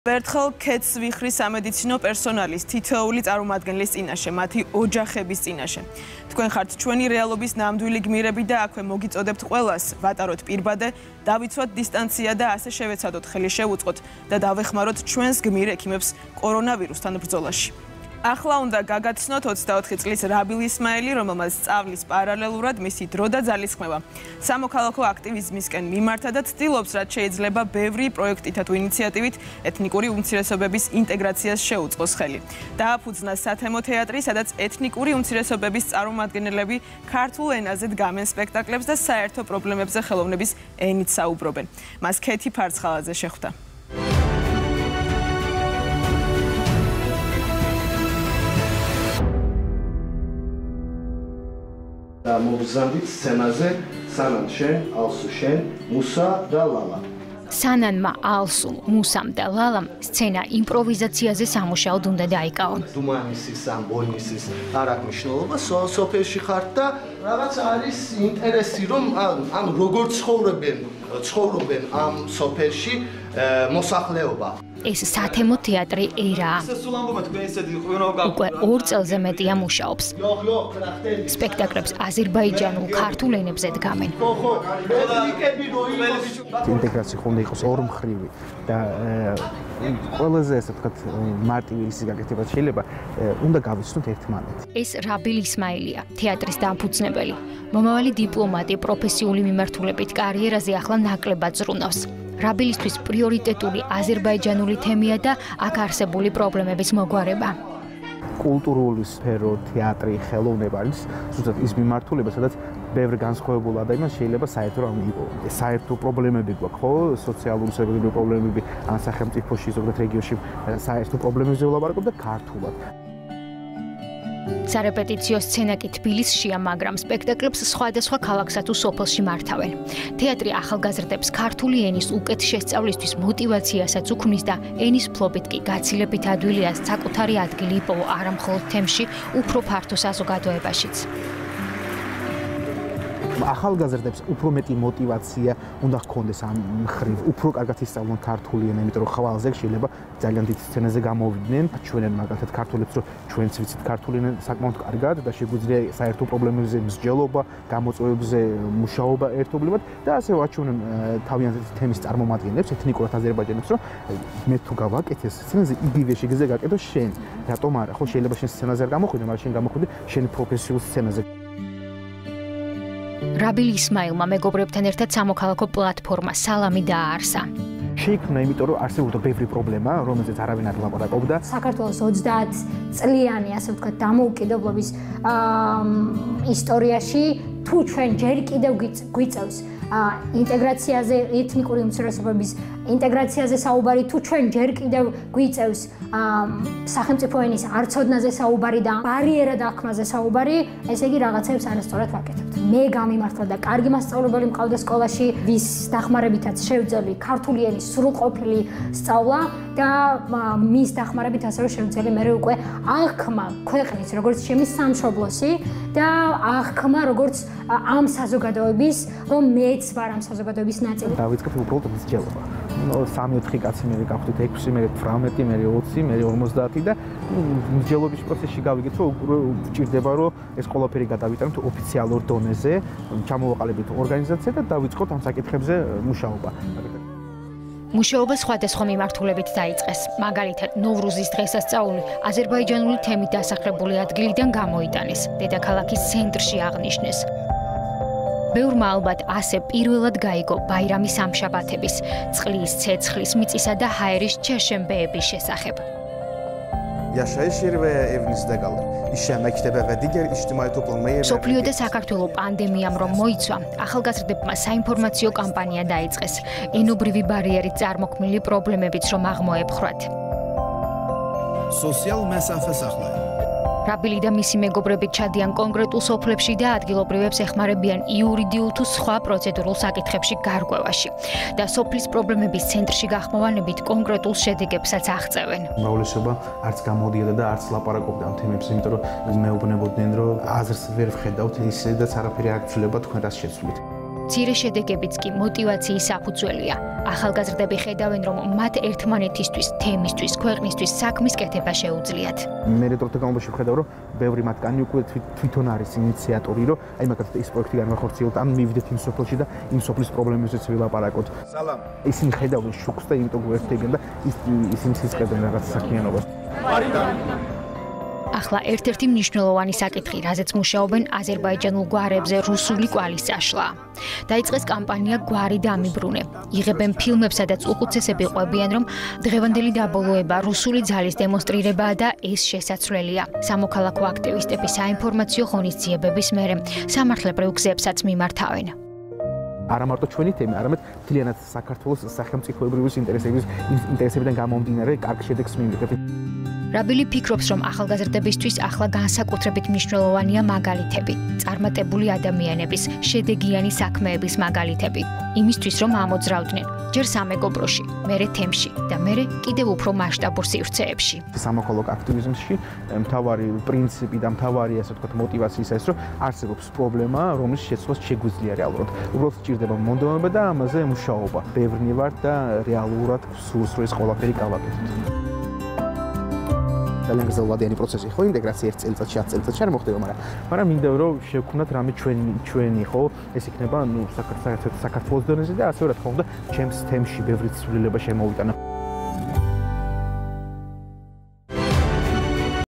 უცნობი გმირი - რაბილ ისმაილი
გაიცანით 24 წლის რაბილ ისმაილი, რომელიც ცდილობს, რაც შეიძლება ბევრი პროექტითა თუ ინიციატივით ეთნიკური უმცირესობების ინტეგრაციას შეუწყოს ხელი. დააფუძნა სათემო თეატრი, სადაც ეთნიკური უმცირესობების წარმომადგენლები ქართულ ენაზე დგამენ სპექტაკლებს